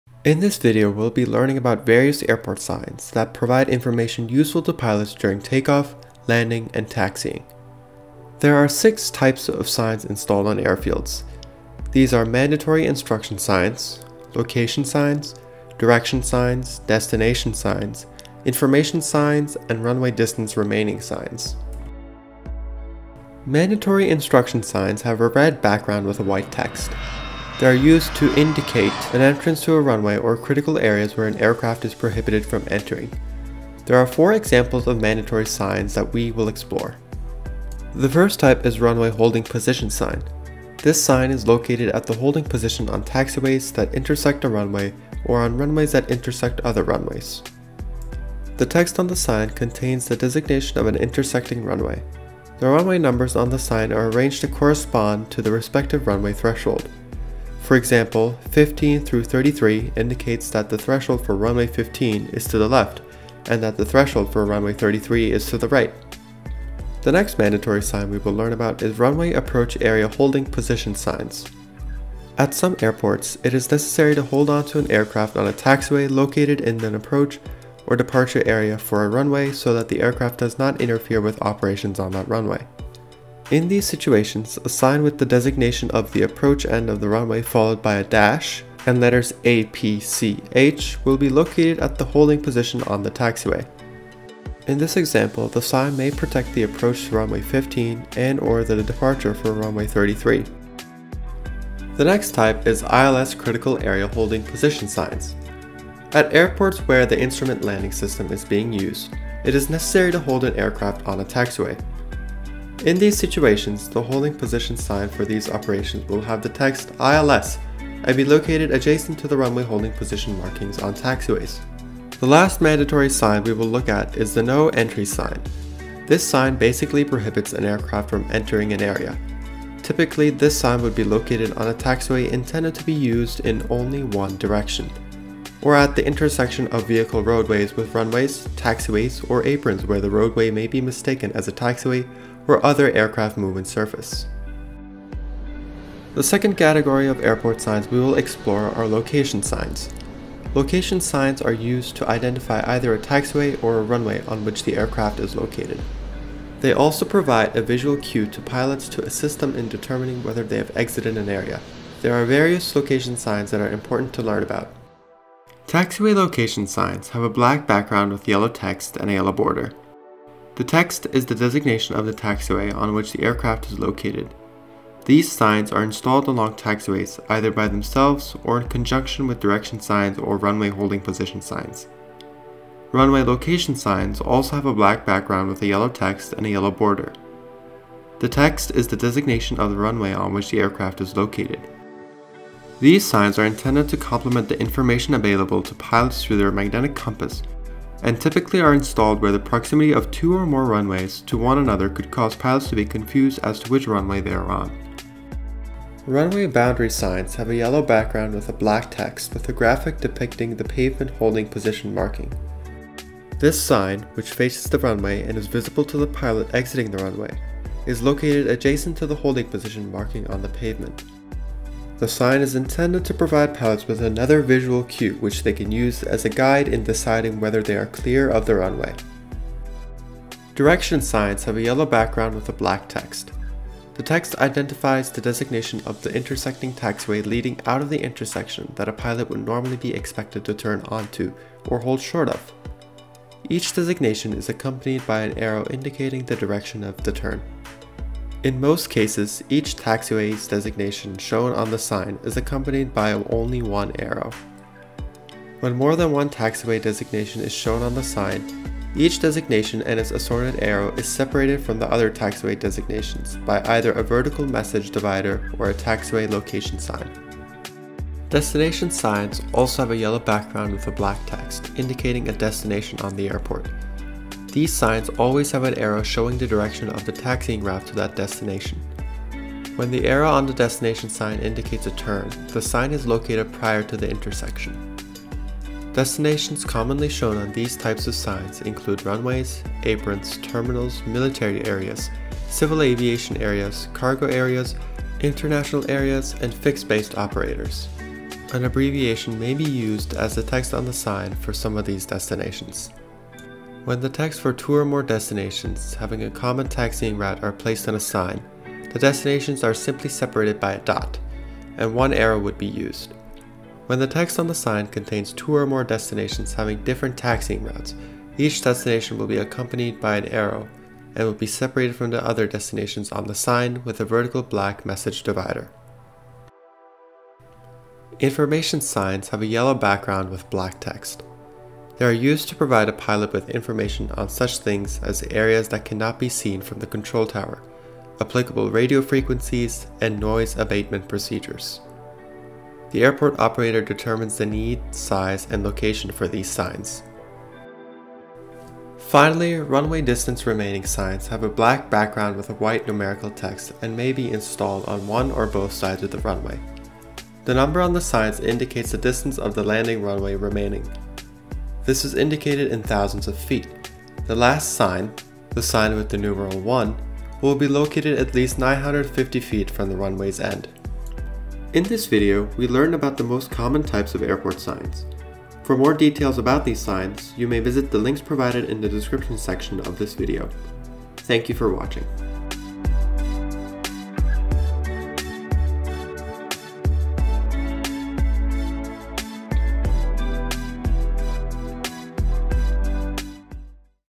Airport signs, explained
Learn about the various signs found in airfields - runways, taxiways, and holding areas.

Part of Youth Aviation Adventure (YAA) Education Series

For more information about these signs and standards, you can review the "Aeronautical Lighting and Other Airport Visual Aids: Airport Marking Aids and Signs" section of the Federal Aviation Administration Aeronautical Information Manual